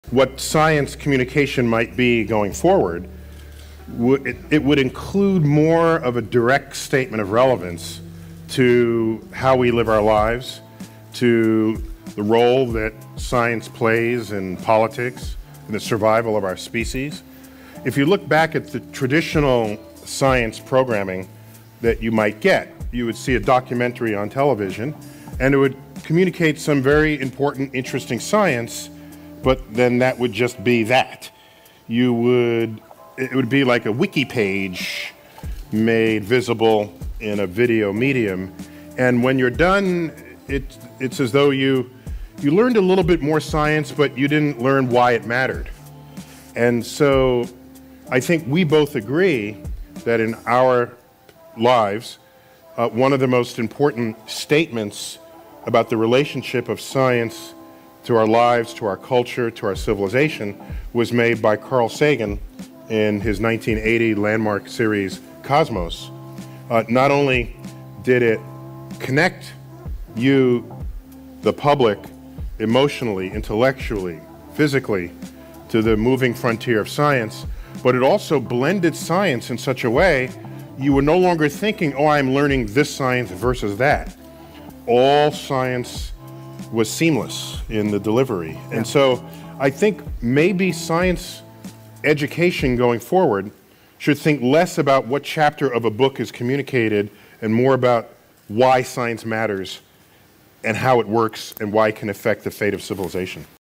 Science Communication Must Evolve to Save Civilization w/ Neil deGrasse Tyson
Science communication is no longer just about facts—it's about relevance. Future science education must connect discoveries to the survival of our species, the shaping of politics, and the fate of civilization itself. Carl Sagan’s Cosmos did more than teach science—it made you feel its urgency. Today, we must return to that emotional, cultural connection and ask not just what is science, but why does it matter?